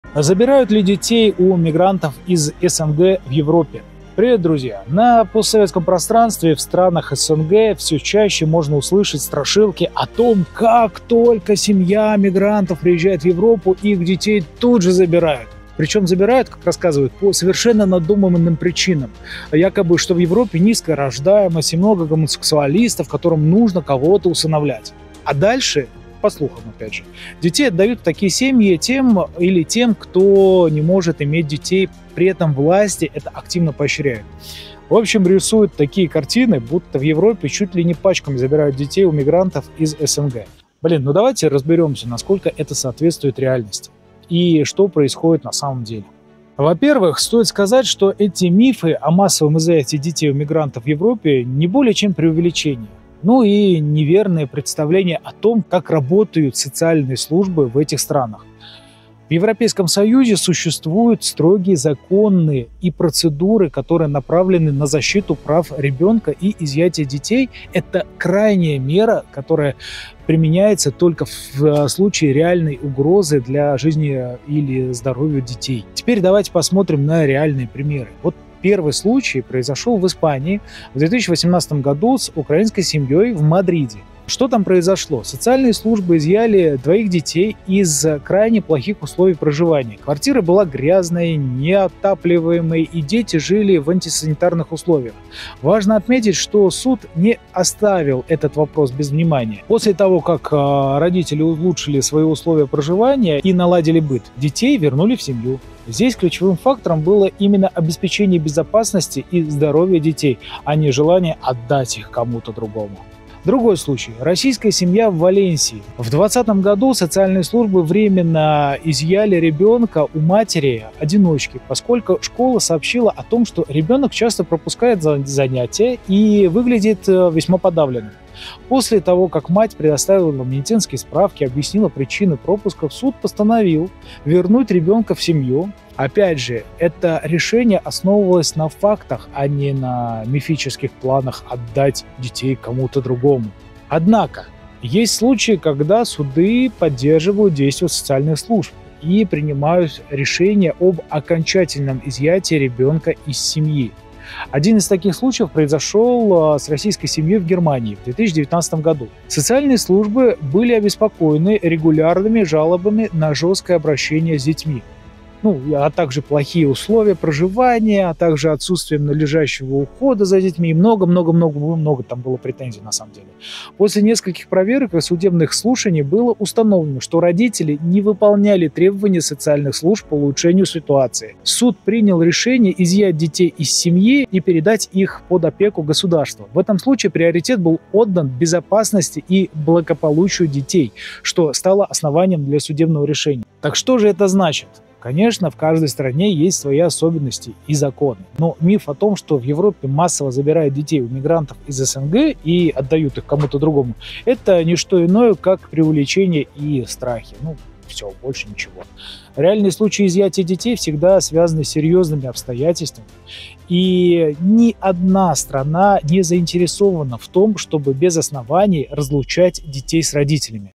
В ЕВРОПЕ МАССОВО ЗАБИРАЮТ ДЕТЕЙ У МИГРАНТОВ?
В этом видео мы разбираем распространенные мифы о том, как в Европе массово забирают детей у мигрантов. Особенно часто такие слухи можно услышать в контексте Германии, России и Испании. Многие верят, что органы опеки в Европе активно отбирают детей у мигрантов и беженцев, чтобы передать их в другие семьи. Но так ли это на самом деле? Мы рассмотрим реальные случаи, когда в Европе забрали ребенка, и узнаем, что на самом деле происходит, когда органы опеки забирают детей. В этом видео вы узнаете правду о том, как и почему забирают детей у мигрантов и беженцев, и почему эти истории чаще всего оказываются сильно преувеличенными. Не пропустите – реальность может вас удивить!

Таймкоды к видео:
0:00 - Забирают ли детей у мигрантов из СНГ в Европе?
0:13 - Мифы о том, что в Европе массово забирают детей
0:42 - Как работают социальные службы в Европе: строгие законы и процедуры
1:16 - Пример: украинская семья в Мадриде, Испания (2018)
1:47 - Пример: российская семья в Валенсии, Испания (2020)
2:19 - Пример: российская семья в Германии (2019)
3:14 - Заключение: мифы о массовом изъятии детей в Европе и реальность